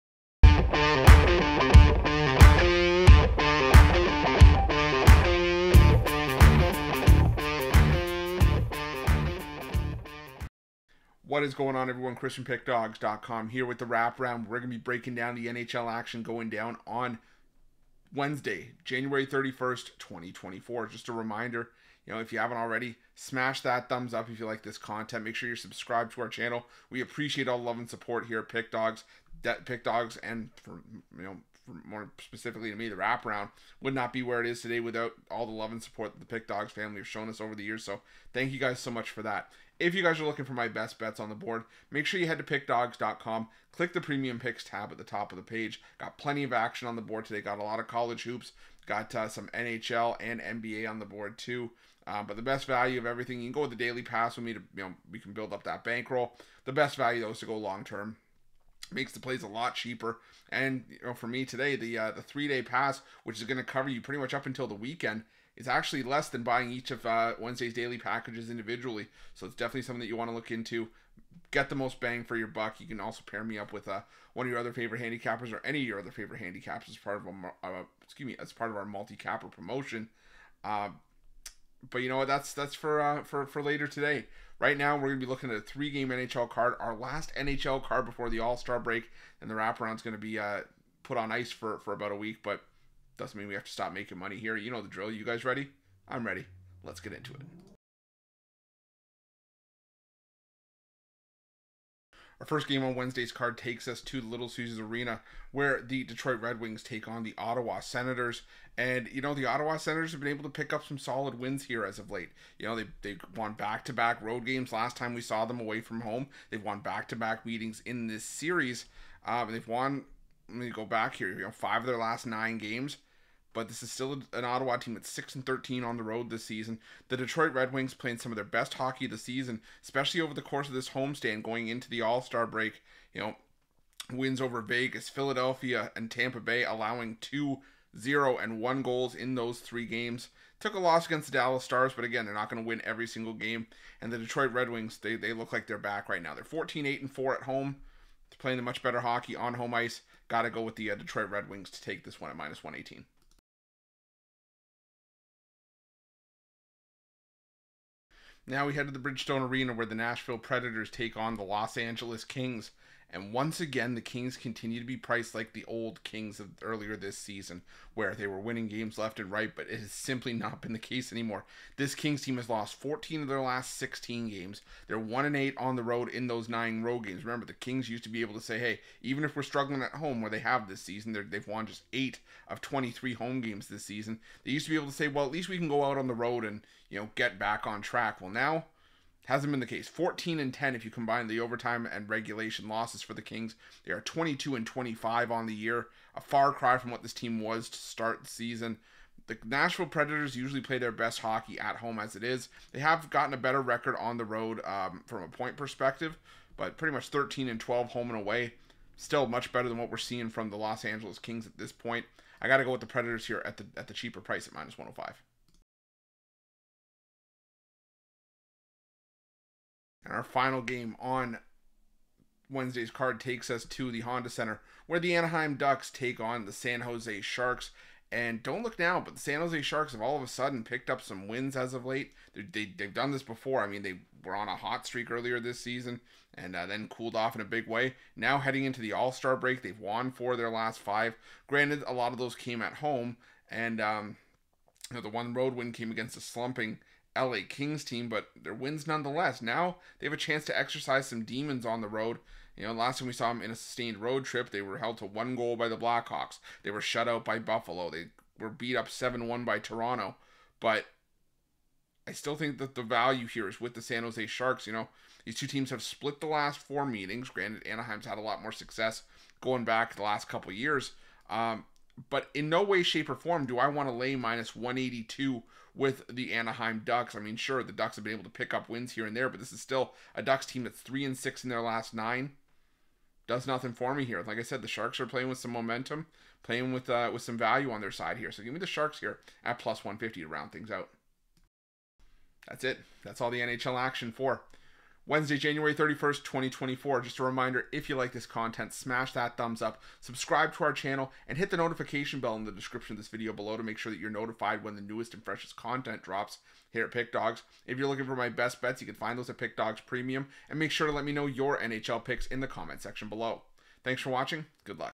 NHL Picks & Predictions Today 1/31/24 | The Wraparound
NHL picks for 1/31/24 Free NHL Picks and Prediction NHL Betting Tips

Chris Ruffolo goes through the NHL schedule on Wednesday, January 31st, 2024, and makes some free betting picks.

Follow Chris on Twitter: @RuffThePickDawg